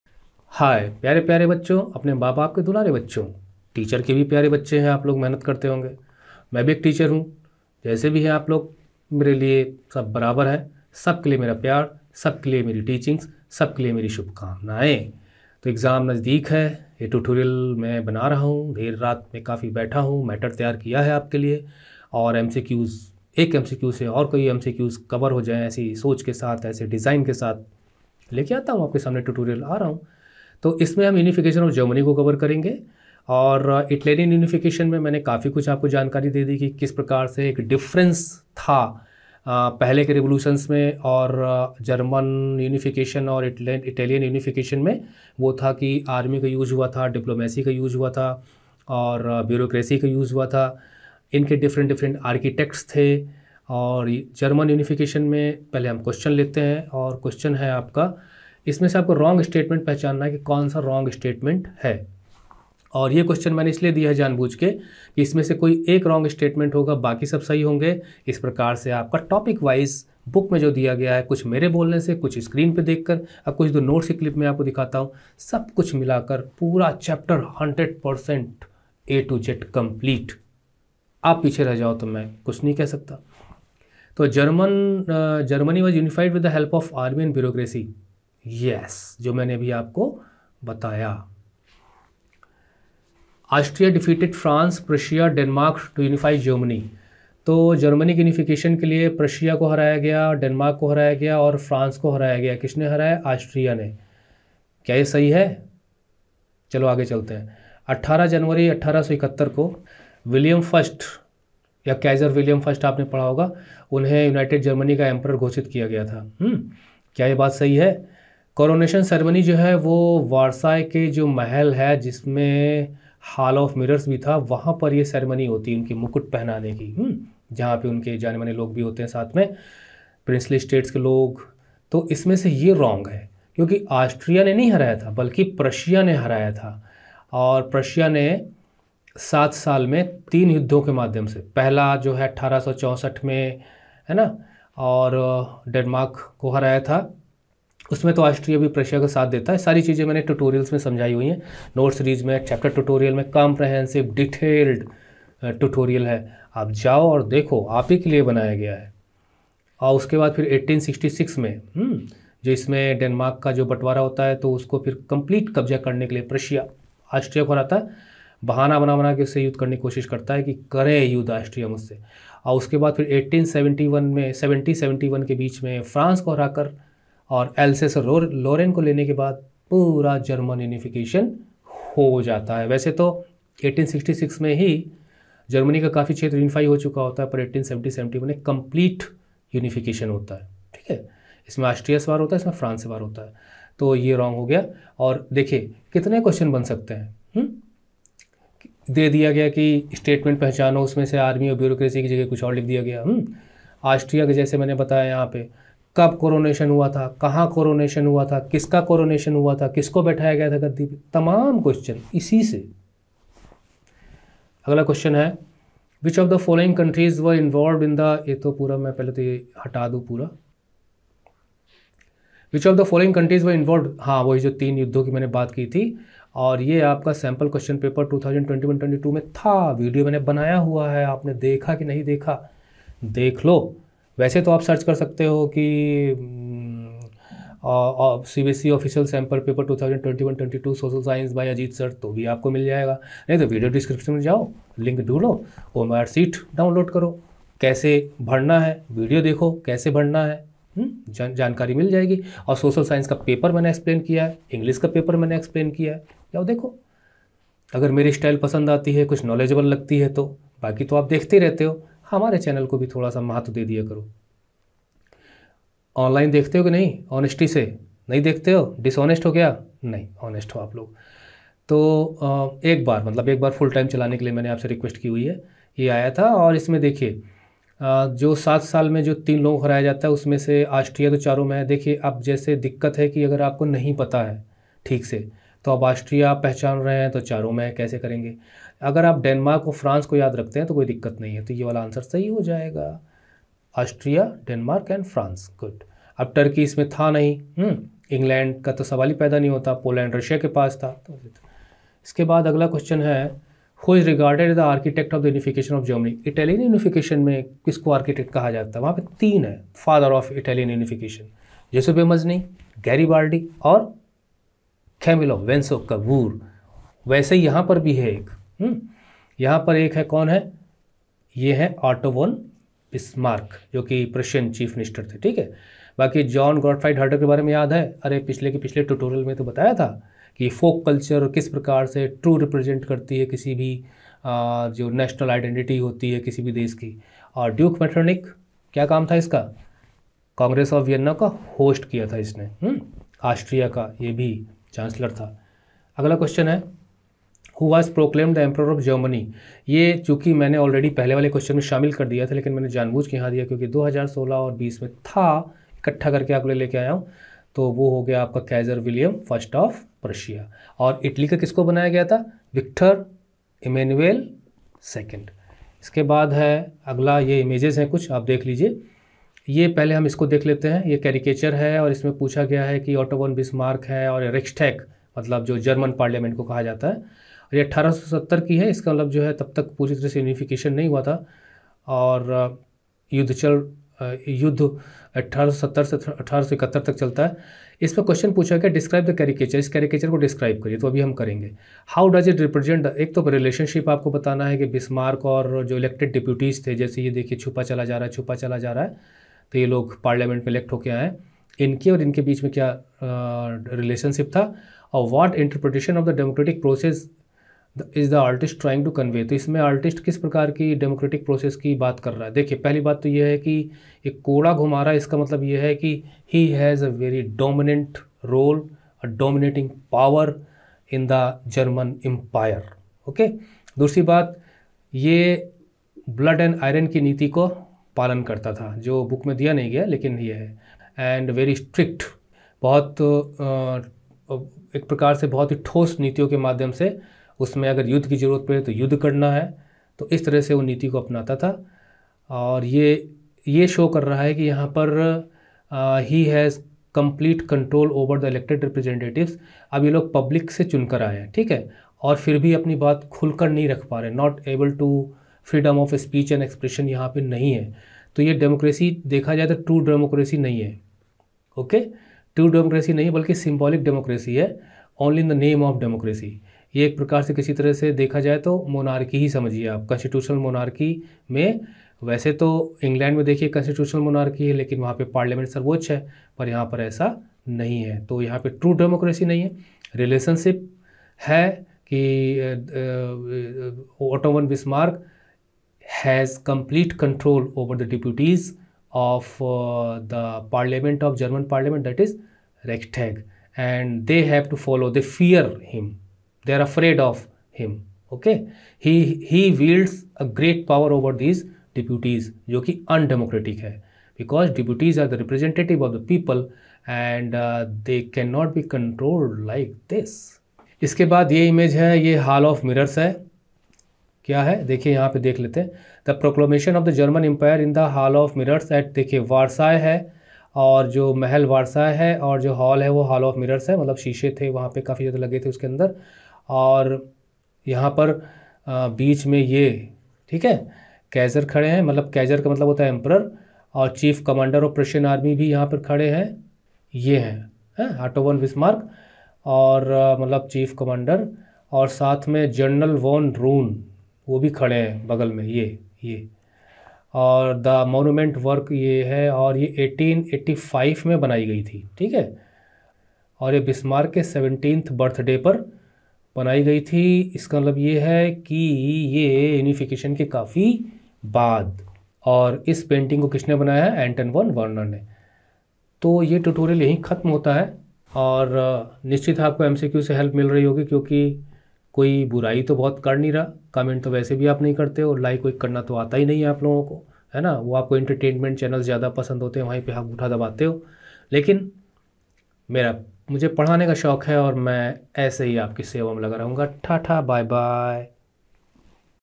MCQs #7 - Nationalism in Europe | Topicwise MCQs | CBSE Class 10 History Term 1 Exam | Ajeet Sir
Topicwise History Class 10 -  Comepetency based MCQs  Chapter ' The Rise of Nationalism in Europe'. Topics - Unification of Germany.
The playlist of the videos and links to study materials are given below.
📖Study Materials - Notes, Solutions etc.